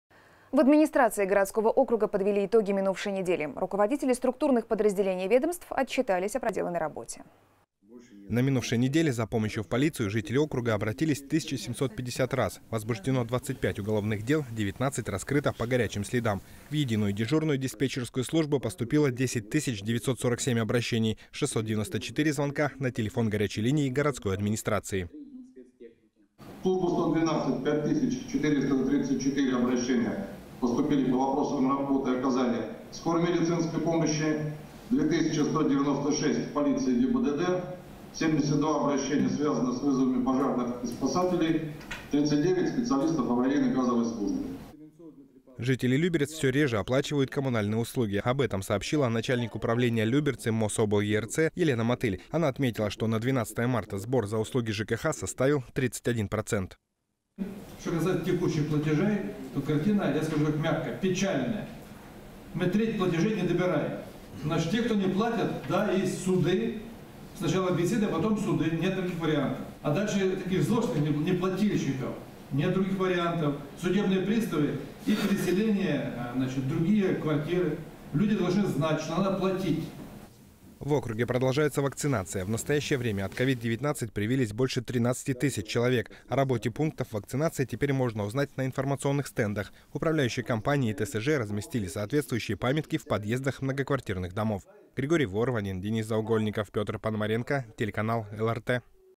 Еженедельное оперативное совещание в администрации
В администрации городского округа подвели итоги минувшей недели. Руководители структурных подразделений и ведомств отчитались о проделанной работе.

На минувшей неделе за помощью в полицию жители округа обратились 1750 раз. Возбуждено 25 уголовных дел, 19 раскрыто по «горячим следам». В единую дежурную диспетчерскую службу поступило 10947 обращений. 694 звонка - на телефон горячей линии городской администрации.

Виталий Богданов, директор МУ «ЕДДС – 112»:
«В службу 112 – 5434 обращения по вопросам оказания скорой и медицинской помощи. 2196 в полицию и ГИБДД. 72 обращения связаны с вызовом пожарных и спасателей. 39 специалистов аварийной газовой службы».

Жители Люберец все реже оплачивают коммунальные услуги. Об этом сообщила начальник управления «Люберцы» МособлЕИРЦ Елена Мотыль. Она отметила, что на 12 марта сбор за услуги ЖКХ составил 31%.

Владимир Ружицкий, глава г.о. Люберцы:
«Что касается текущих платежей. Я скажу мягко говоря картина печальная. Мы треть платежей не добираем. Значит те, кто не платят, есть суды. Сначала беседы, а потом суды. Нет других вариантов. А злостных неплательщиков, нет других вариантов: судебные приставы и переселение в другие квартиры. Люди должны знать, что надо платить».

В округе продолжается вакцинация. В настоящее время от КОВИД-19 привились больше 13 тысяч человек. О работе пунктов вакцинации теперь можно узнать на информационных стендах. Управляющие компании и ТСЖ разместили соответствующие памятки в подъездах многоквартирных домов.